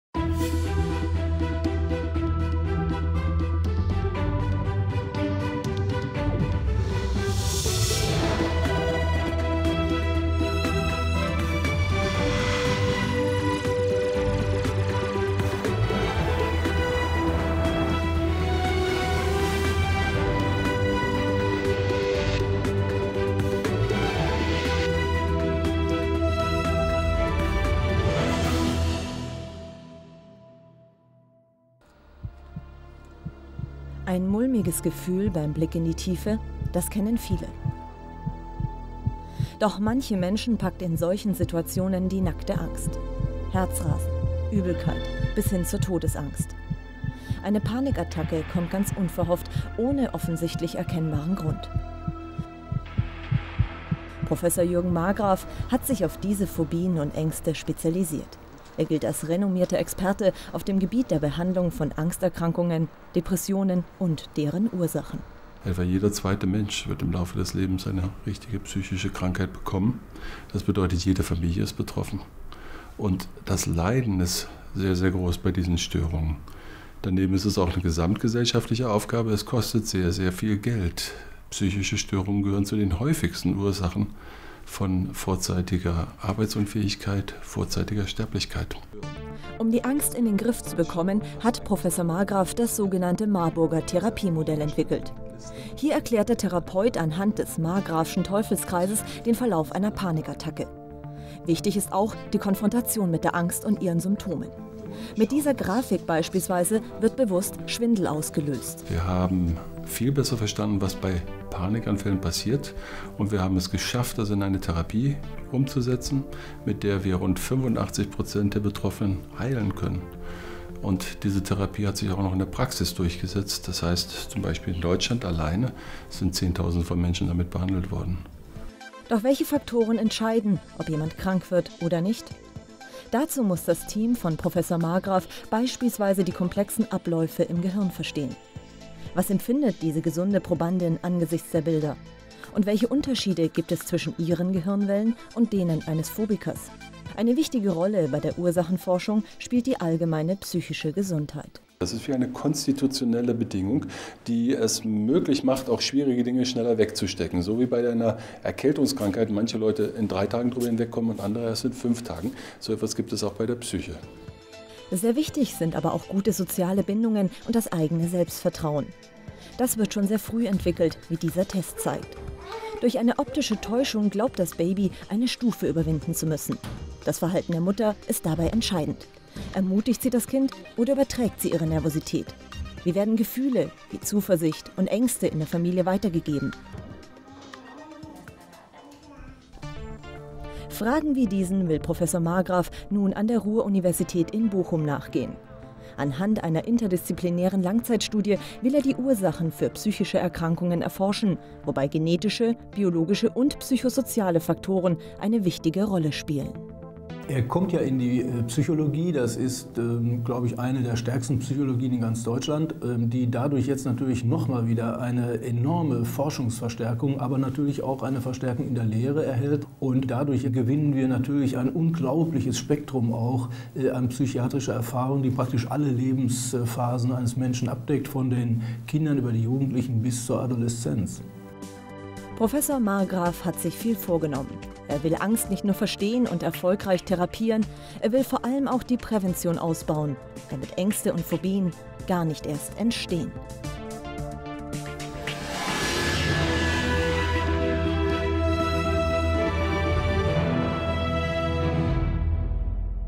Jürgen Margraf - Alexander von Humboldt-Professur 2010 (DE)
Psychologie
Jürgen Margraf gilt als führender Experte auf dem Feld der klinischen Psychologie und der Psychotherapie. Einer seiner Schwerpunkte ist die Therapie von Angststörungen. Er ist ein Verfechter von kurzen und auf ihre Wirksamkeit hin evaluierten Behandlungsformen und beriet in diesem Sinne als Mitglied des Wissenschaftlichen Beirates für Psychotherapie die deutsche Bundesregierung. Seine jüngsten Arbeiten zielen darauf, Bedingungen für eine gute psychische Gesundheit zu identifizieren, um so Erkrankungen vorbeugen zu können. An der Universität Bochum soll Margraf den in den letzten Jahren entwickelten Forschungsschwerpunkt Klinische Psychologie stärken und das geplante neue Forschungs- und Behandlungszentrum für Psychische Gesundheit leiten.